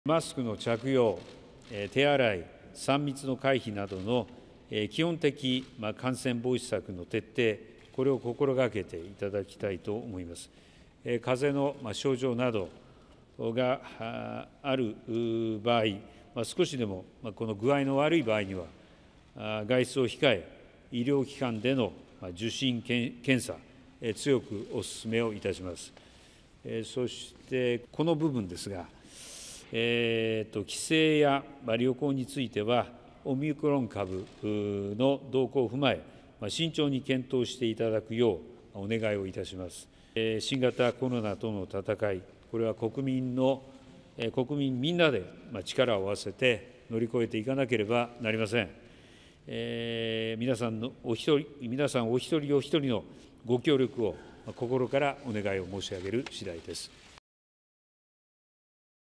年末年始における国民の皆様へのお願い
年末年始にあたり、マスクの着用、手洗い、３密の回避などの基本的な感染防止策の徹底を心掛けて頂きたいと思います。風邪の症状などがある場合は、少しでも具合の悪い場合には外出を控え、医療機関での受診・検査、強くお勧めします。
そして、帰省や旅行については、オミクロン株の動向を踏まえ、慎重に検討してください。新型コロナとの闘いは国民皆で力を合わせて乗り越えていかなければなりません。皆さんお一人おひとりの御協力を心からお願いを申し上げます。